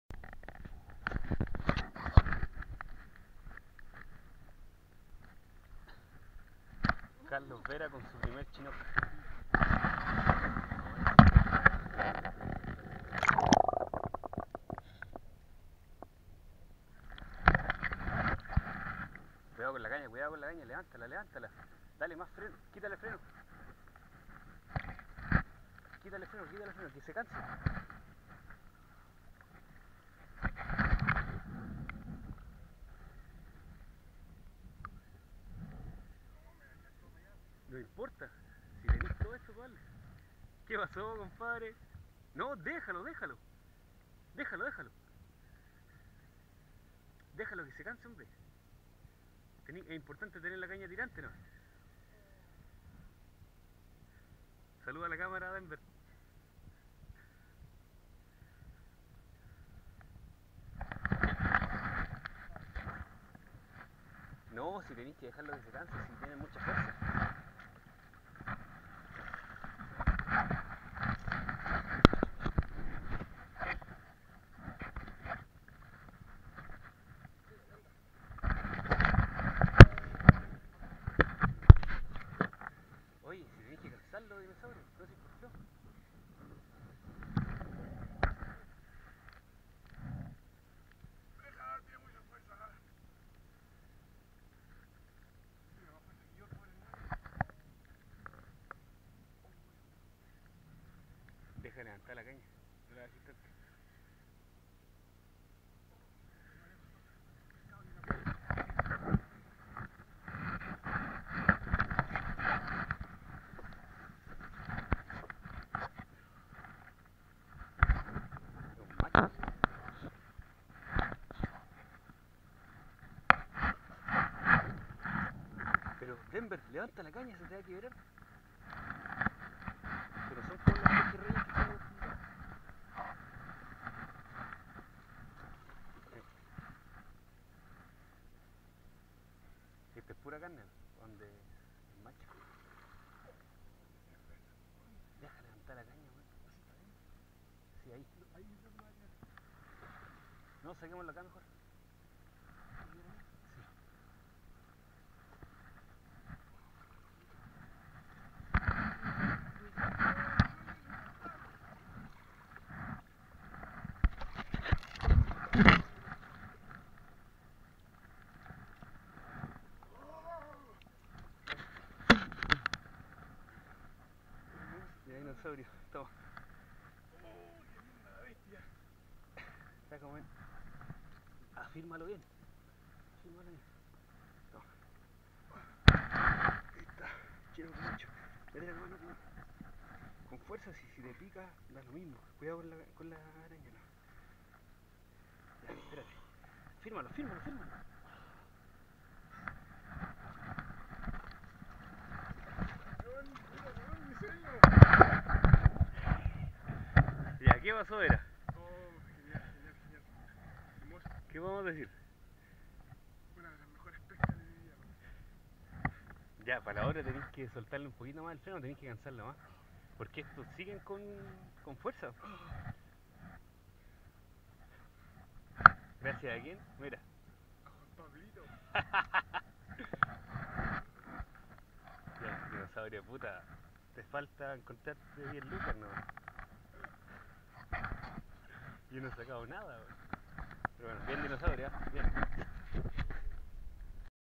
Pesca salmón chinook Puerto Montt
Dinosaurio pescanduo salmón chinook, febreruo 2020 Puerto Montt.